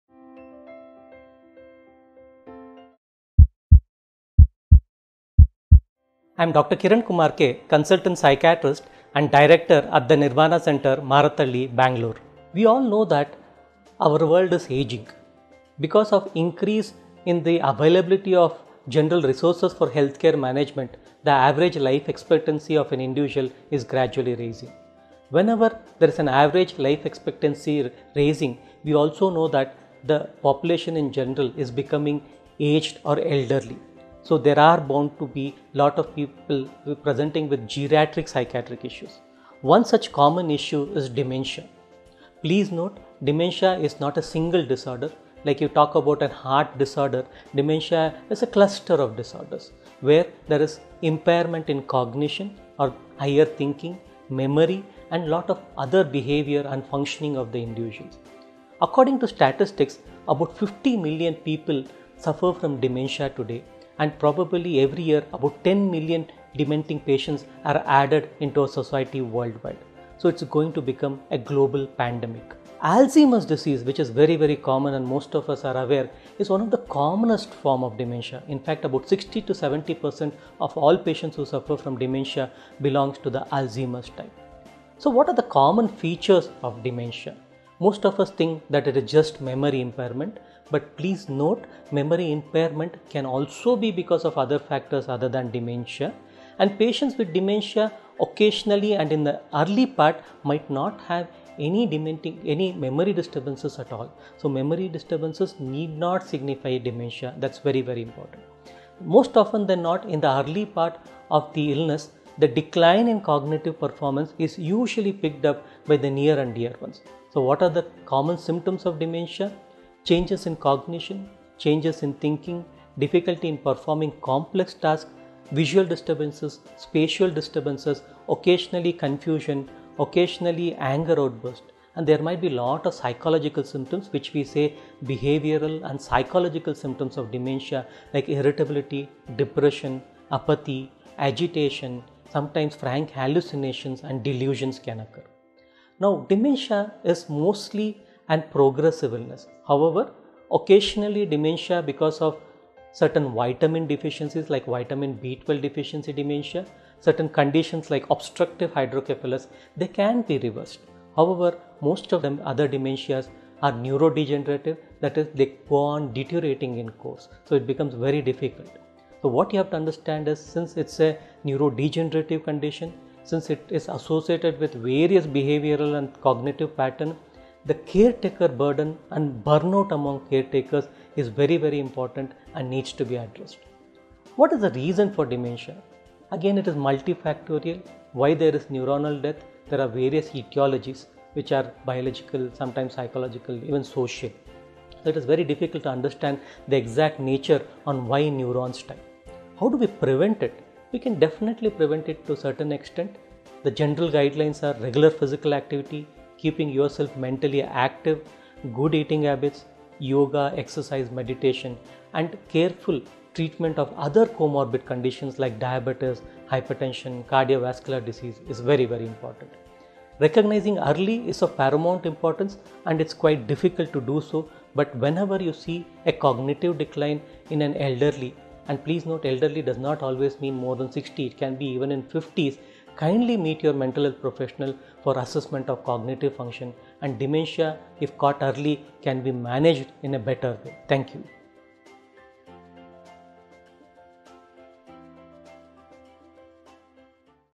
Old Age | Manage patients with Dementia | Alzheimer's Disease - Dr. Kiran Kumar | Doctors' Circle
Dr. Kiran Kumar K | Appointment booking number: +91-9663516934
Consultant Psychiatrist & Director | The Nirvana Center, Marathahalli, Bangalore
The average life expectancy  is  raising and the population in general is becoming  aged  and hence  more of  geriatric  psychiatric  issues dementia  is  not  a  single disorder, it is a  cluster of  disorders, there is behavioral changes. According to statistic of 50 million people suffer  from dementia.  It is  a  global pandemic. Alzheimer’s disease  is   commonest  form of  dementia.  What are  the common features of  dementia?  Memory  problems  can also   be due  to other  factors  other than  dementia. There can  be  memory  disorders  or  not.  What  are  the  common symptoms  of  dementia? There is change in  cognition,  change in thinking.